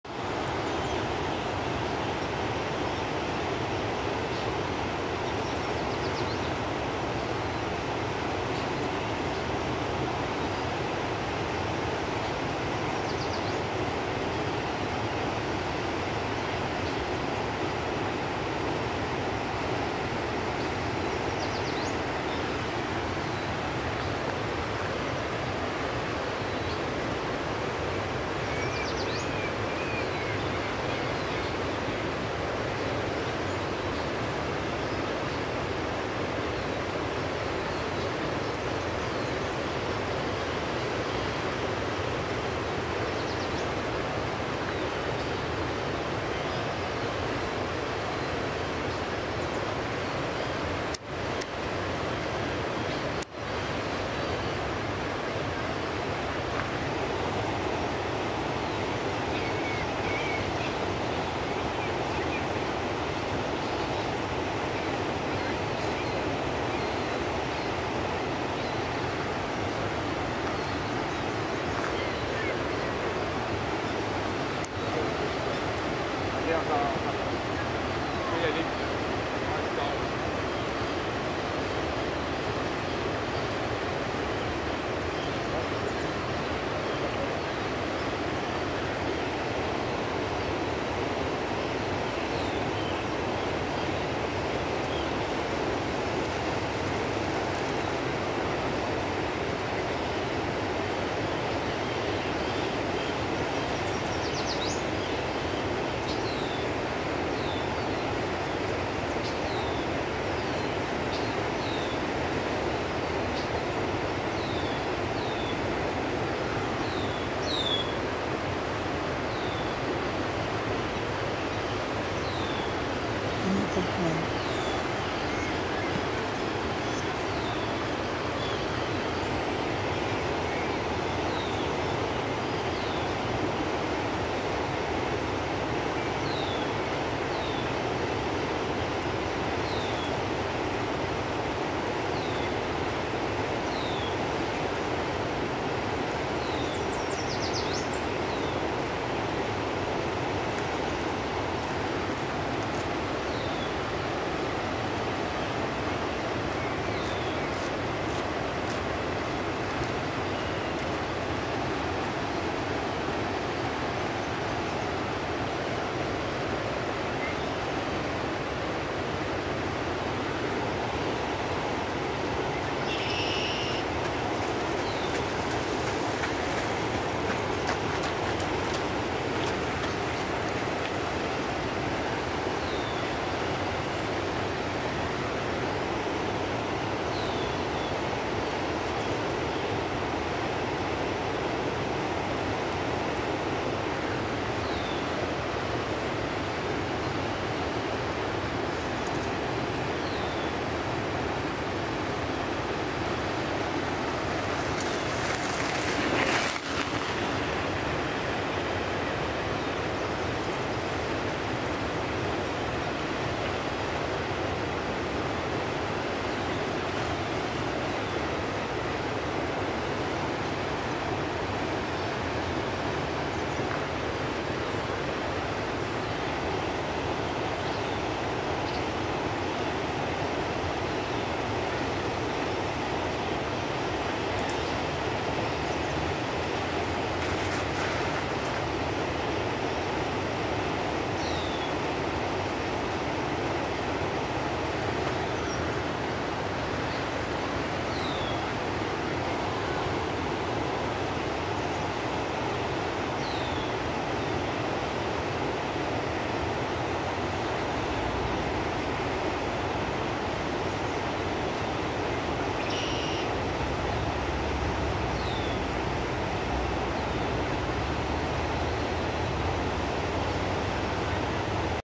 Tranche de vie d'une petite colonie de sternes pierregarins - 3 juillet 2020
Sur les berges de Verdun:
- 2 bébés à gauche près du chevalier grivelé (durant la première minute)
- 2 bébés presqu'au centre, près du tronc
- 3:30: bébé à gauche nourrit
- 3:45: adulte près du tronc nourrit